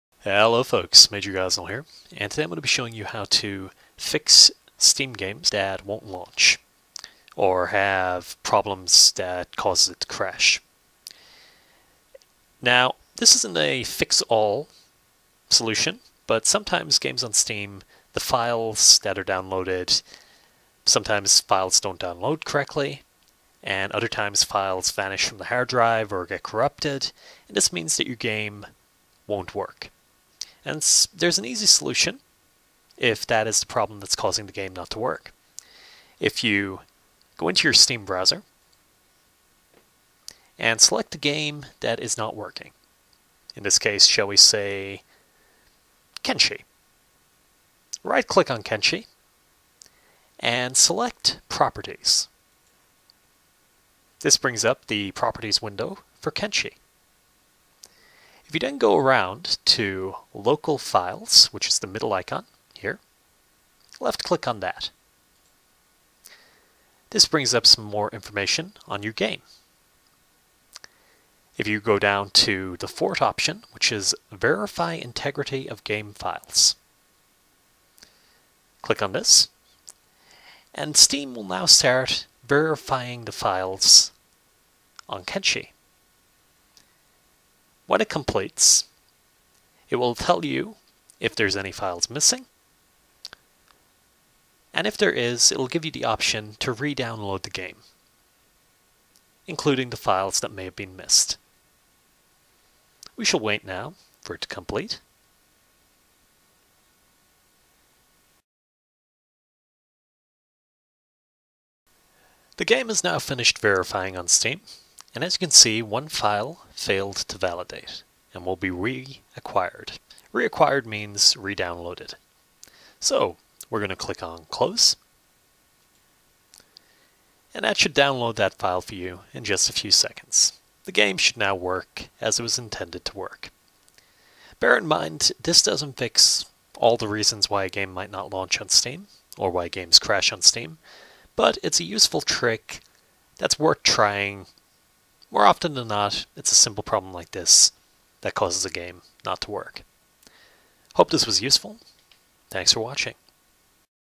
Steam Guide:How To Fix Broken Games On Steam
A quick guide to fixing Games not launching on Steam